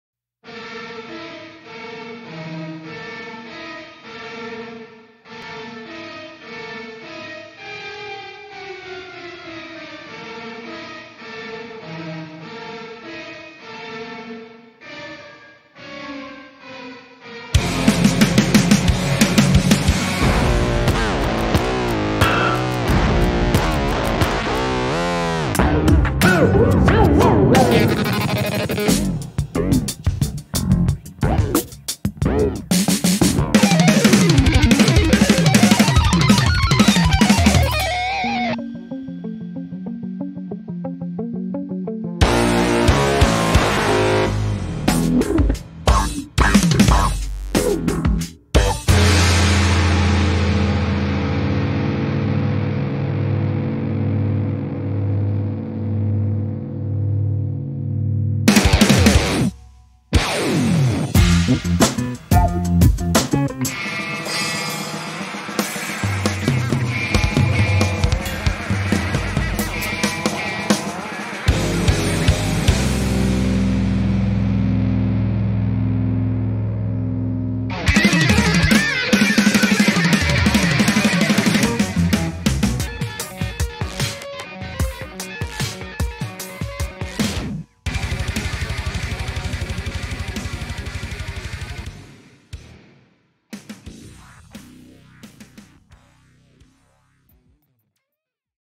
Original Signed Canvas Paintings by Buckethead for sale on the Pikes Kiosk 🎨
New paintings added to the bottom of the page! Woo!!!